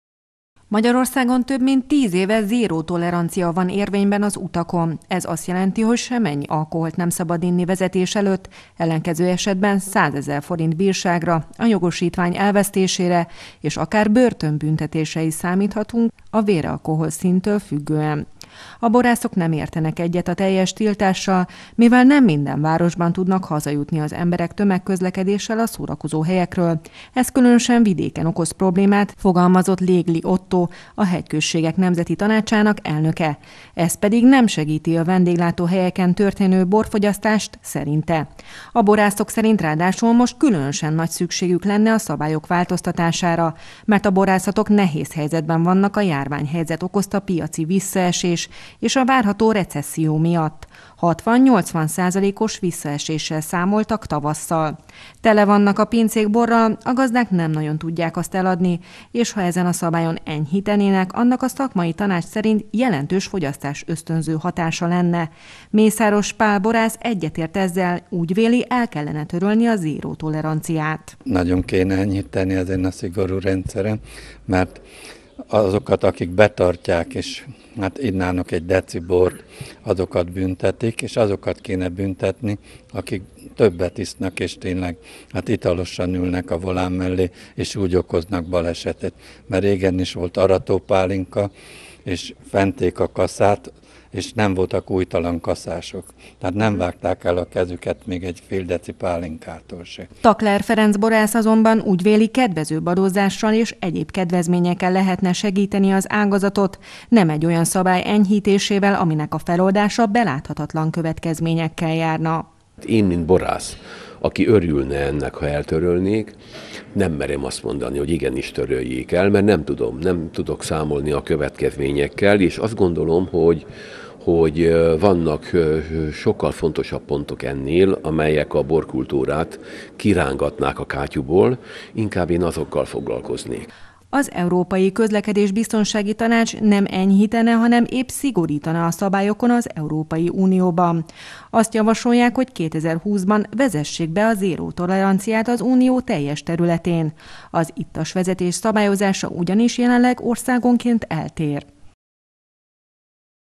Zéró tolerancia - a borászok egy része enyhítene a szigorú közlekedési szabályokon 20200622
Lehessen inni egy pohár bort az ebédhez és utána autóba ülni avagy sem?  Újra előkerült a zéró tolerancia eltörlése. Légli Ottó, a Hegyközségek Nemzeti Tanácsának elnöke úgy véli, enyhíteni kellene a szabályokon. A szekszárdi borászokat megosztja a téma. A rendőrség pedig azt emelte ki, hogy jelenleg Magyarországon tilos az alkoholfogyasztás vezetés előtt. Ellenkező esetben száz ezer forintos bírsággal, jogosítvány elvesztésével és akár börtönnel is sújtható az ittas sofőr.